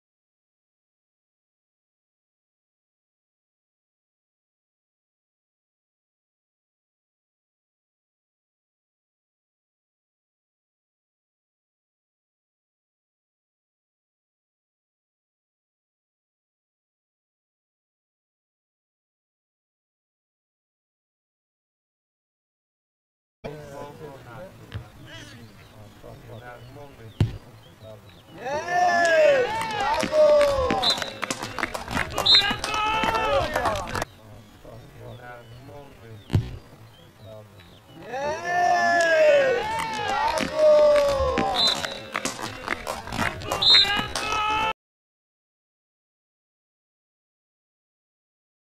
Jacek Guzik 0:6 (69') 14 kolejka A klasy: Jaworzanka - Płomień (04.05.2014r .)
Szósta bramka dla Płomienia w meczu A klasy Limanowskiej: Jaworzanka Jaworzna - Płomień Limanowa. Strzelcem gola Jacek Guzik.